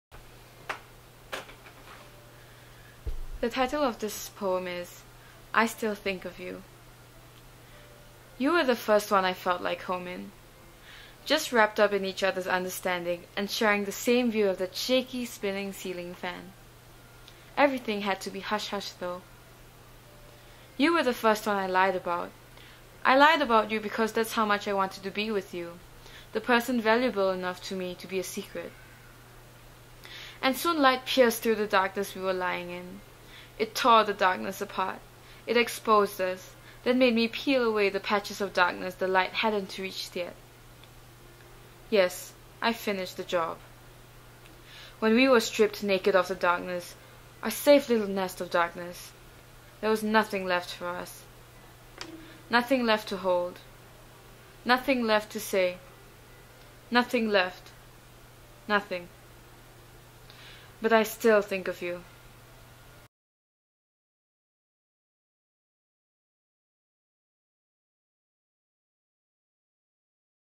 Storytelling at UWC-USA: I Still Think Of You
Storytelling by Heather Hong.
Starring by Tomomi Shiotani.
Directing by Naré Harutyunyan.